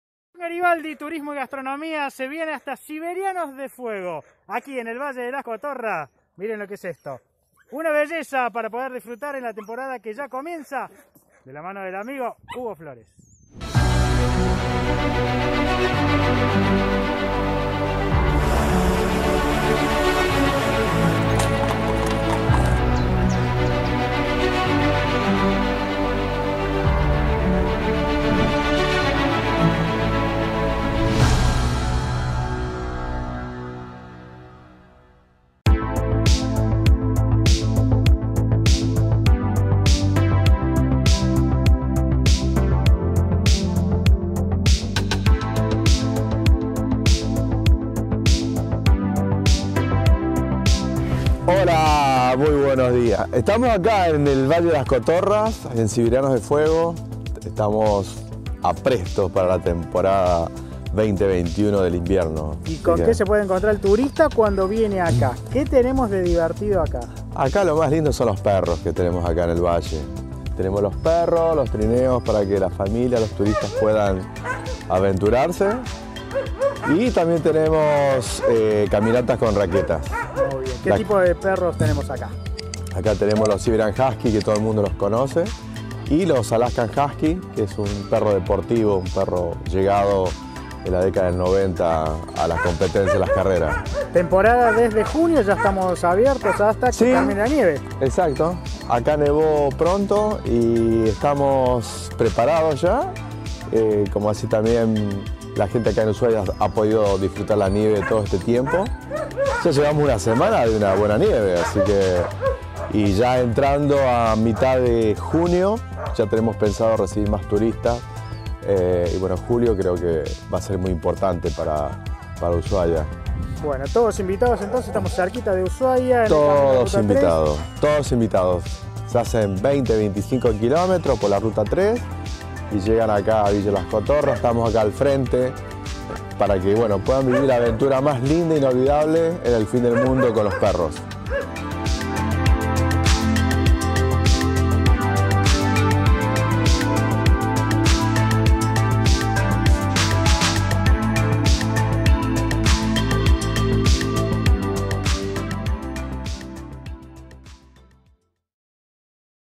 Siberianos De Fuego
El placer de pasear en trineo en "Siberianos de Fuego"
A 25 km de Ushuaia por la Ruta Nacional N°3, en el centro Centro Invernal “Villa Las Cotorras”, se encuentra "Siberianos de Fuego". Con la amabilidad de Hugo Flores, podés conocer  los más de 140 perros nórdicos  Siberian Husky, Alaskan Husky y Samoyedos. Sentir la fuerza y la energía de estos animales, el sonido del trineo deslizándose sobre el sendero nevado, y atravesar la quietud del bosque bajo las ramas desnudas. Un paseo que te conecta directamente con la naturaleza fueguina.
Estos tienen una duración promedio de 20 minutos y el trineo es conducido por un guía que dirige a los perros. Animales nobles y fuertes, resistentes a las bajas temperaturas y bellos para encuadrar las mejores fotos de una experiencia única.